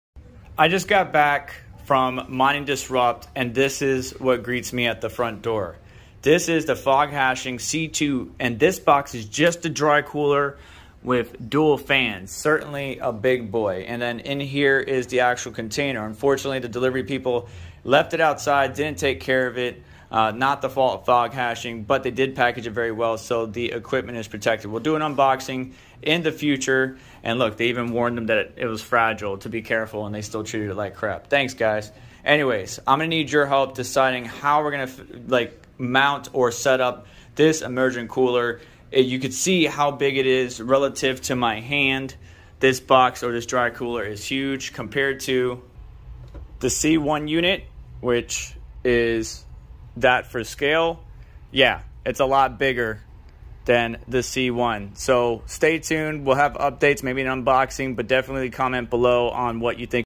Foghashing C2 Kit arrived right after Mining Disrupt 2023
We have shown the Foghashing C1 kit on the channel, but the C2 is HERE. I didn't expect it to be this big (that's what she said) this will be a challenge... We need to figure out the best way to set up the C2 Dry cooler at home.

🔥I got Merch now, Cryptee is hosting SerpentX Merch!!🔥

:::Send Me Stuff:::
SerpentX Tech - Vega
1403 Dunn Ave Ste 2